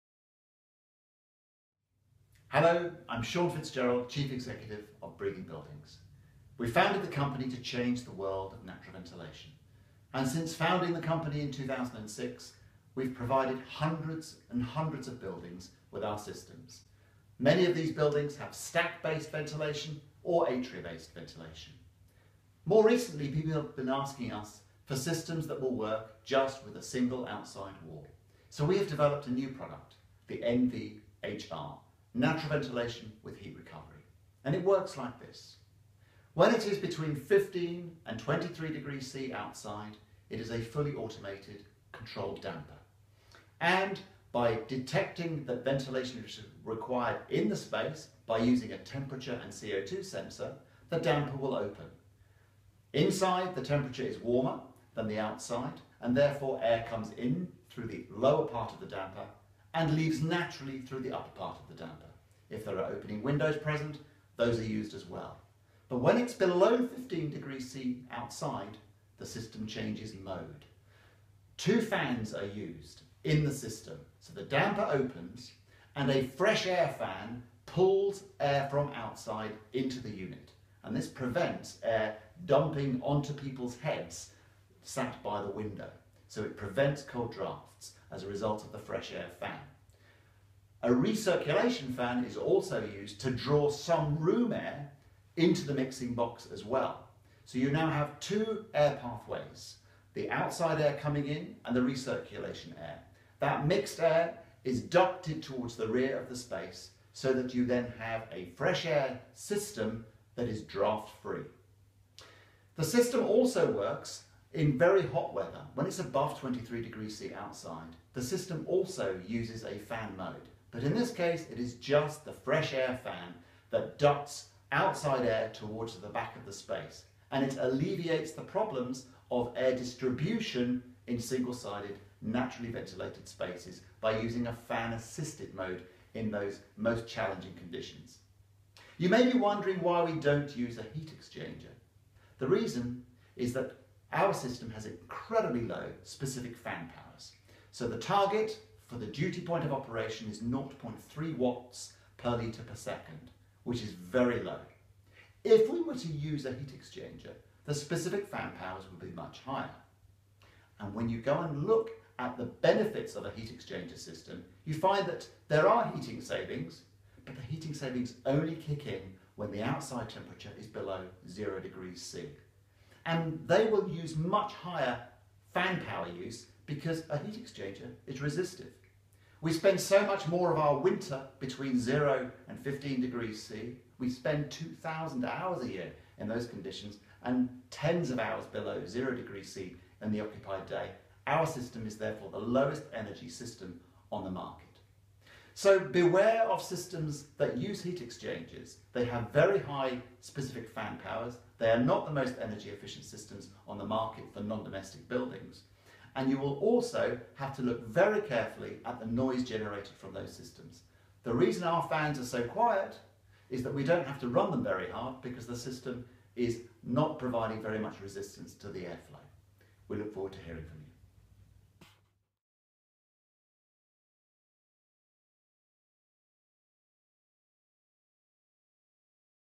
NVHR Video
Shaun Fitzgerald explains how the NVHR from Breathing Buildings is changing the way people think about Natural Ventilation.